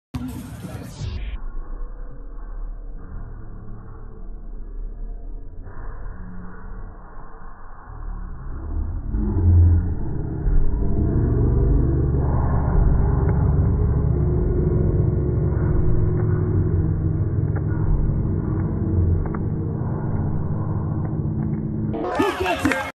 Los Oliver & Benji del futvoley: ¡locura absoluta de punto!
Estos dos jugadores firman un puntazo increíble. Ambos suben la pierna hasta una altura increíble para terminar bloqueándose el mate.